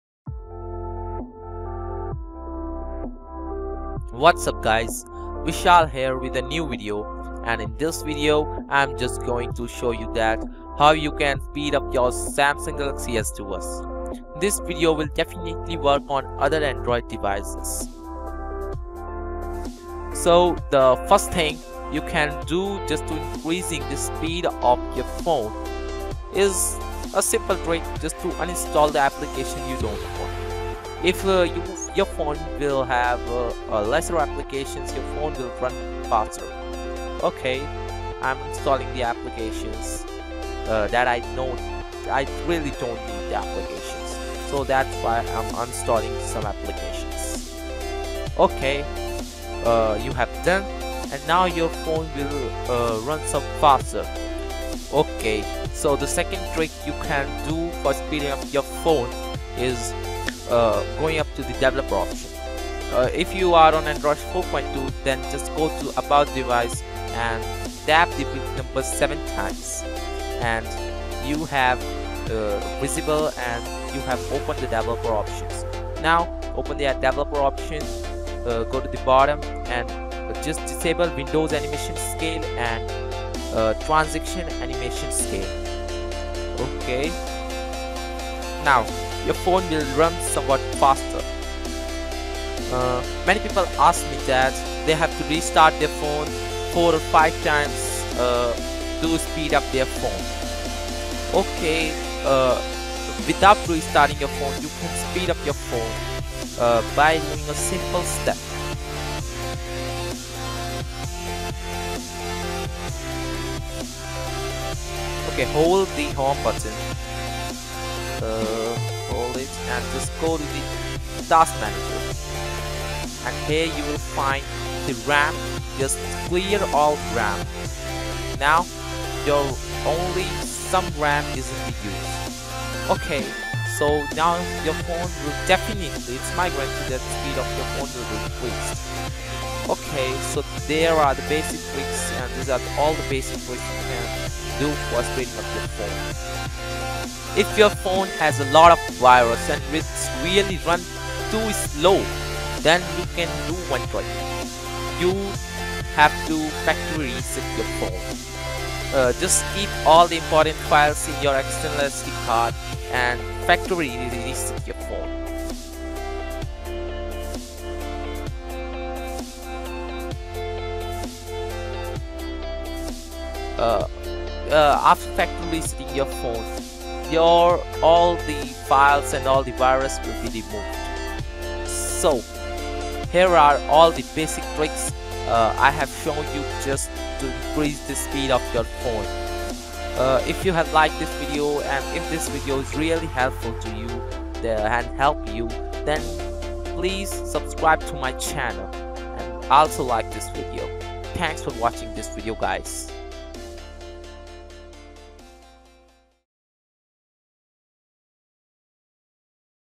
How to speed up Samsung Galaxy S Duos or 2 (S7562 - S7582) (Basic Methods)
How to speed up Samsung Galaxy S Duos or 2 (S7562 - S7582)